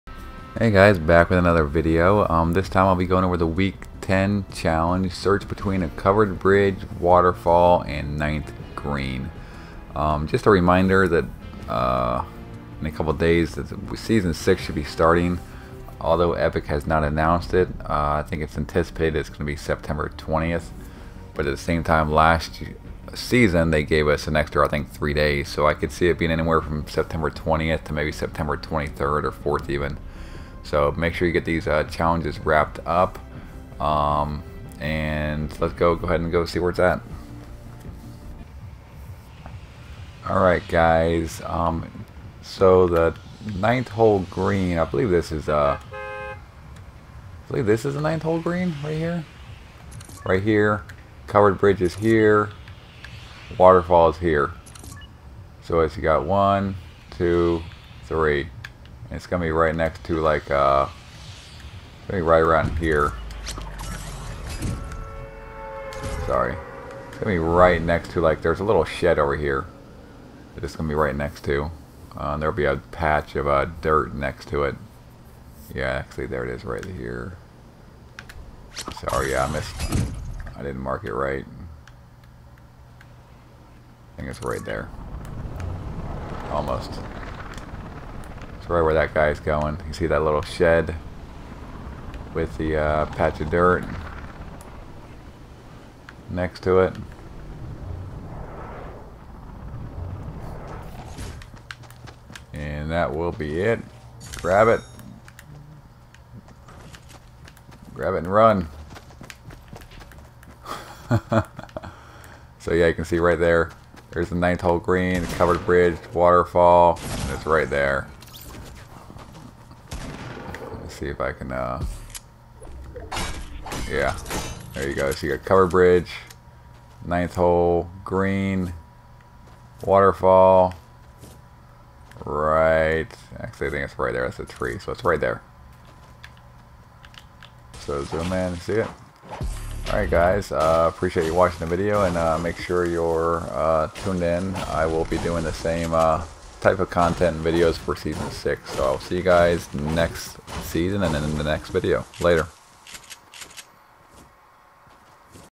Search between a covered bridge, waterfall, and the 9th green
In this video I will show where to find the Week 10 Season 5 Battle Star for the "Search between a covered bridge, waterfall, and the 9th green" Challenge.